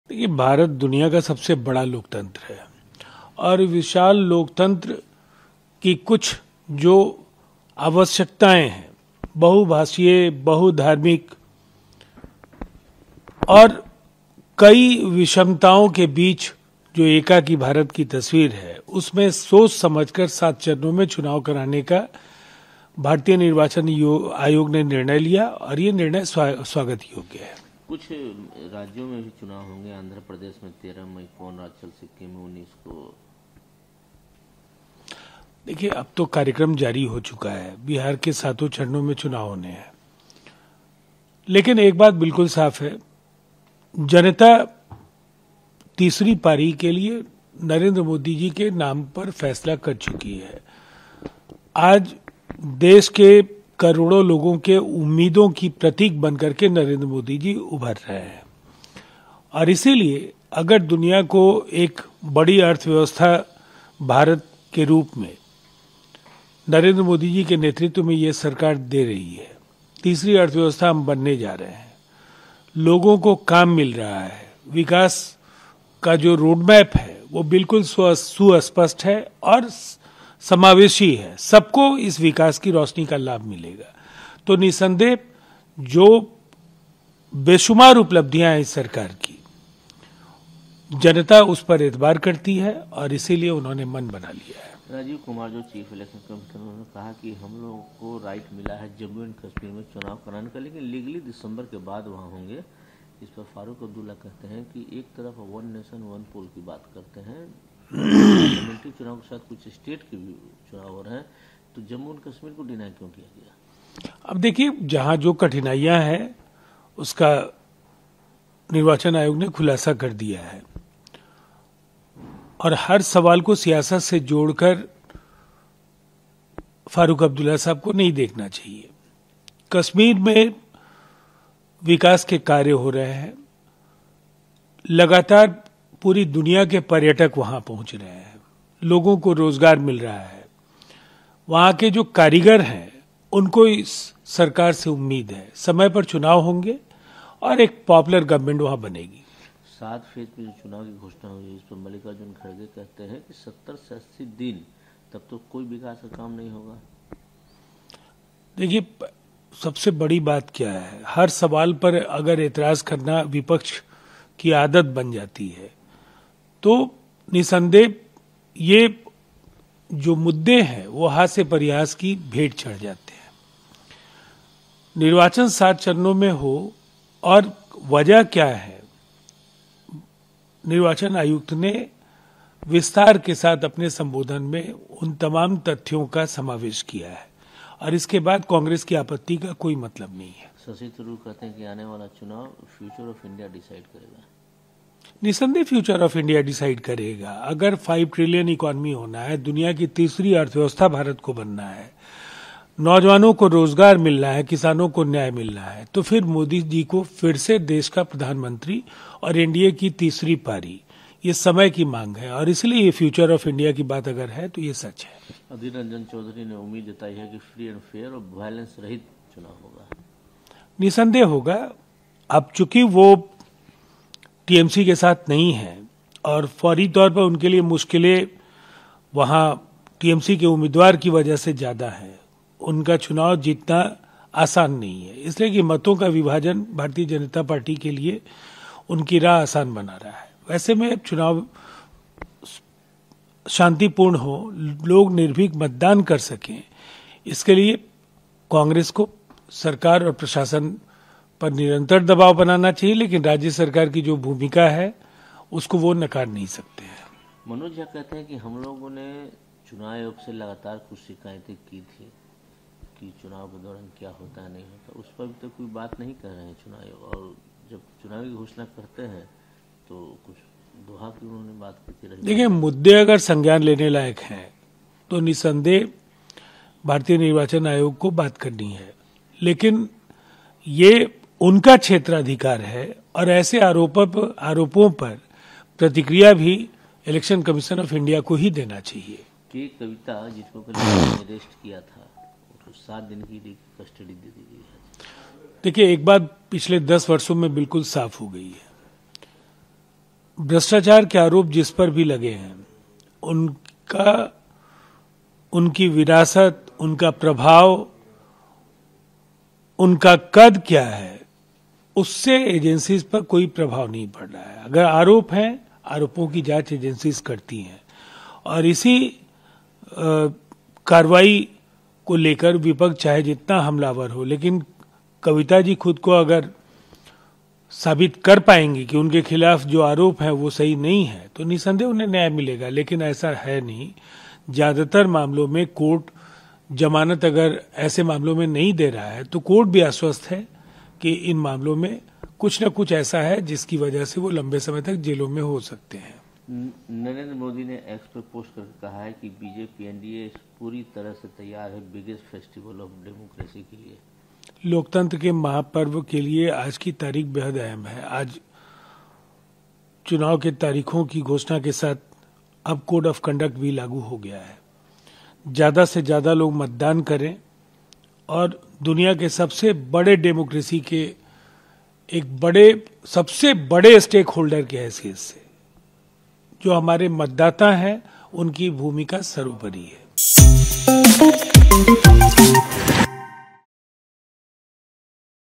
Loksabha Elections: 7 चरणों में चुनाव होने को लेकर विपक्ष की बयानबाजी पर क्या बोले JDU नेता
Loksabha Elections: 7 चरणों में चुनाव होने को लेकर विपक्ष की बयानबाजी पर क्या बोले JDU नेता राजीव रंजन